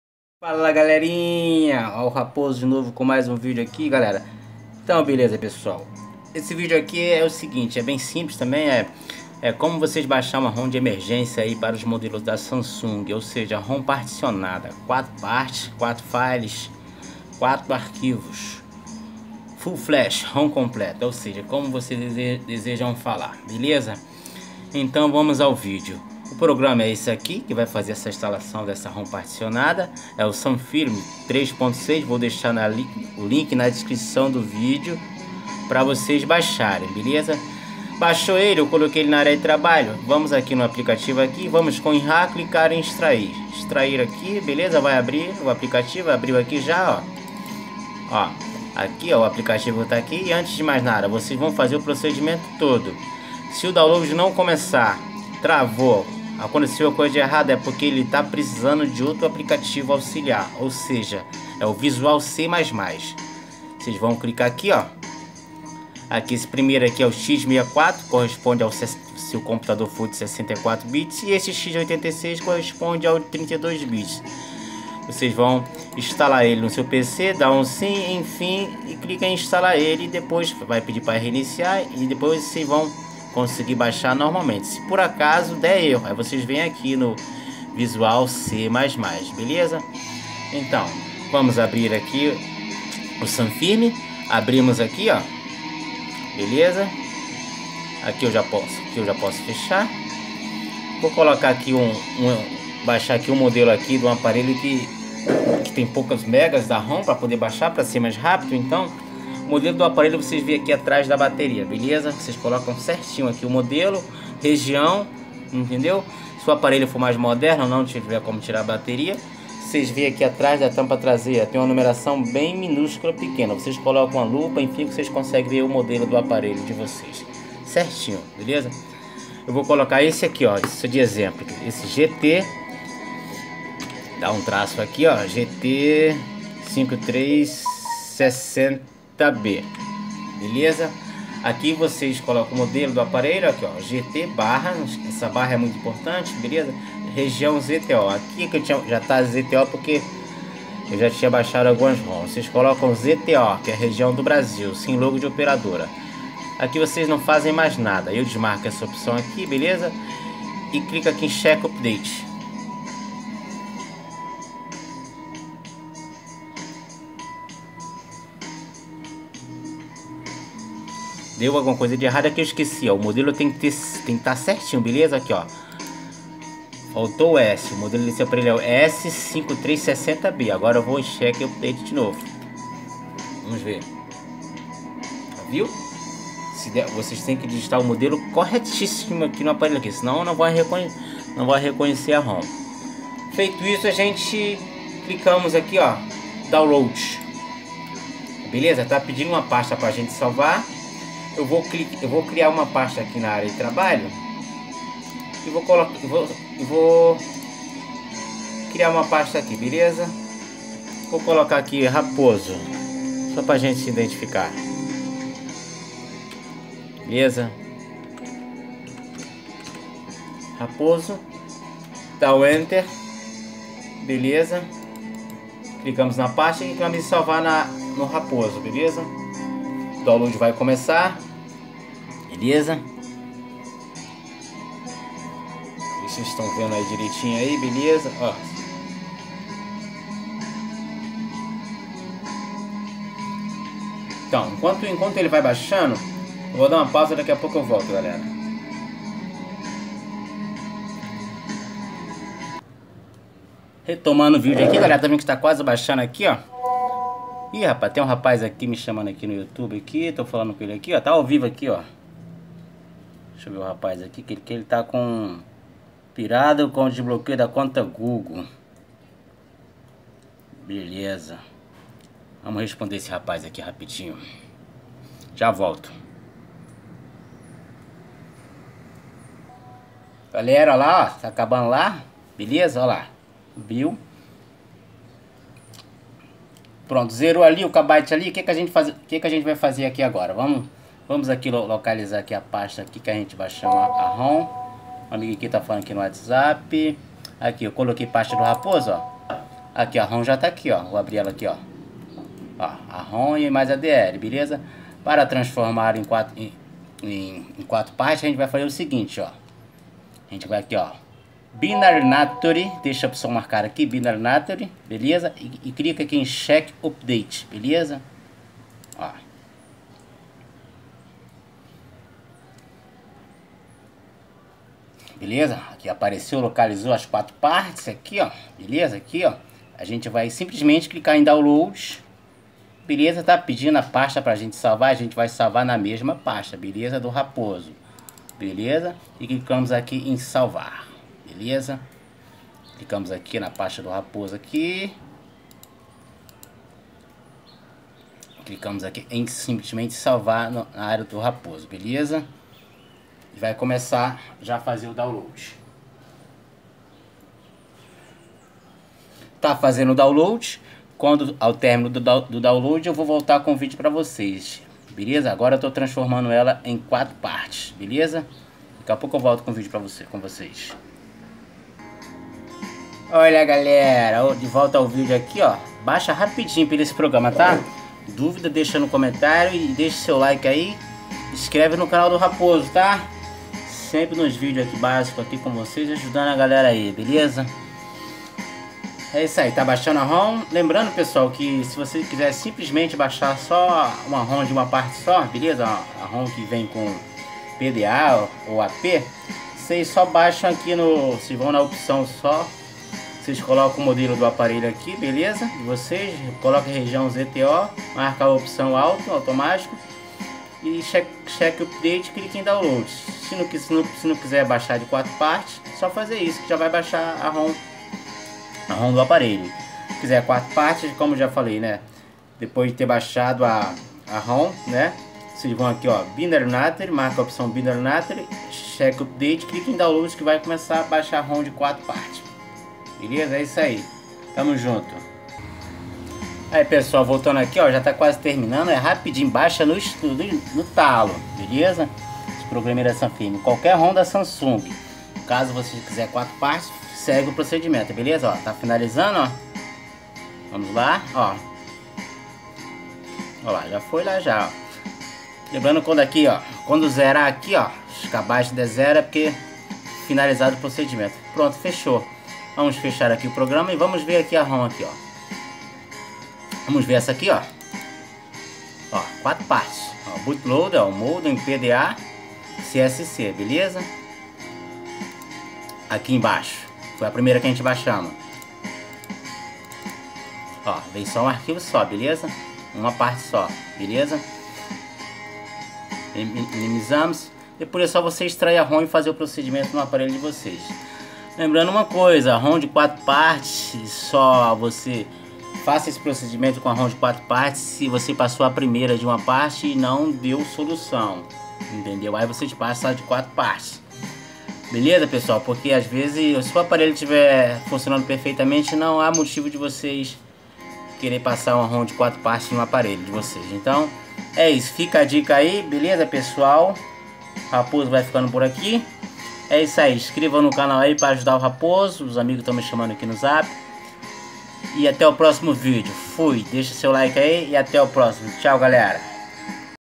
Como baixar roms particionada full Flash 4 arquivos roms de emergência modelos samsung
Como baixar roms particionada full Flash 4 arquivos modelos samsung roms de emergência

Obg pessoal tmj